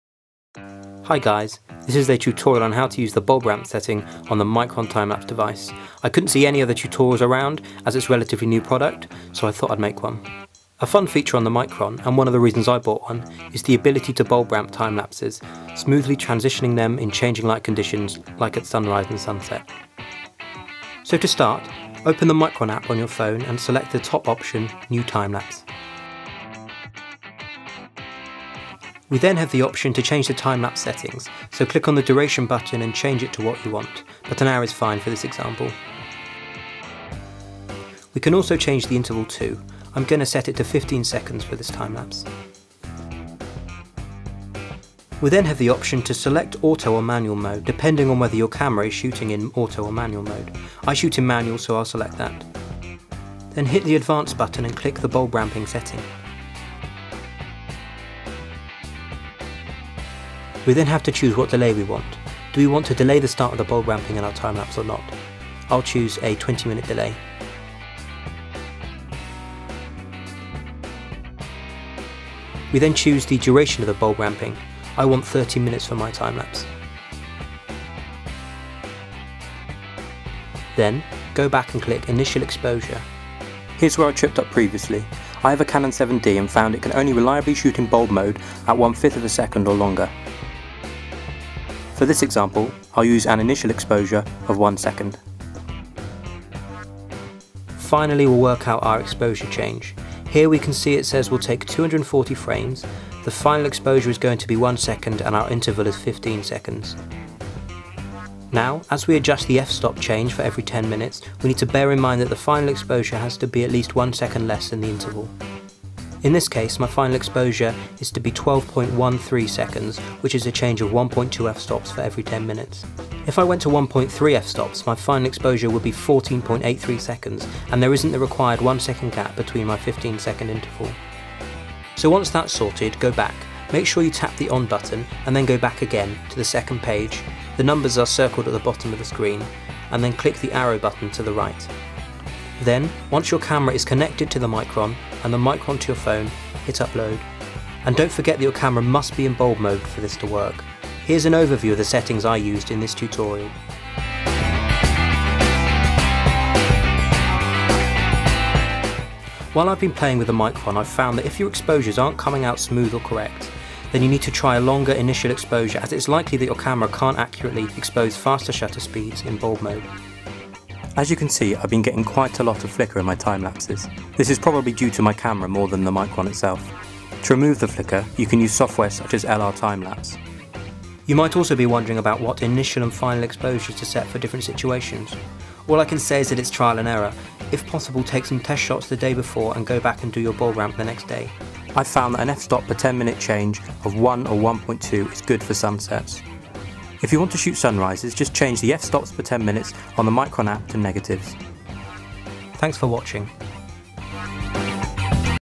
Michron Bulb Ramping Bramp Timelapse Tutorial
In this tutorial I'll be demonstrating how to use the bulb ramp setting on the new Michron device by Vivo Labs. With it, you can produce holy grail time-lapses as well as interesting exposure changes. It's quite tricky at first, but with practice you can get your head round it.

I've included some sunset examples at the end, with the settings too. As I say in the video, there's quite a bit of flicker but you can fix that with LR Timelapse

I've also found that some frames appear dark and mess the sequence up a bit. This is most likely - although I'm not certain - a camera issue and an inaccuracy in bulb mode. I'd be interested to know if others are having the same problems!

Nonetheless, it's a great tool. Check out my tutorial for interval ramping with the Michron too